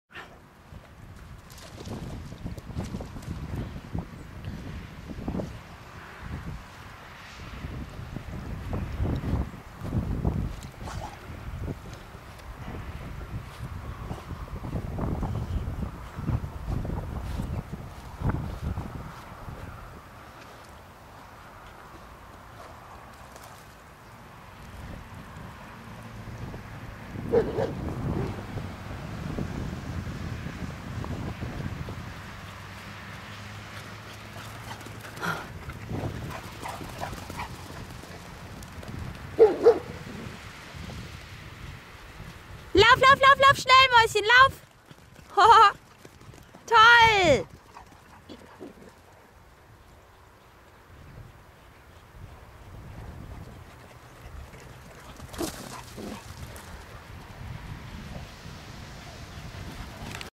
Hundebetreuung in meiner Hundepension in Schleswig-Holstein
BB spielt den Hasen für Wonnie und Digga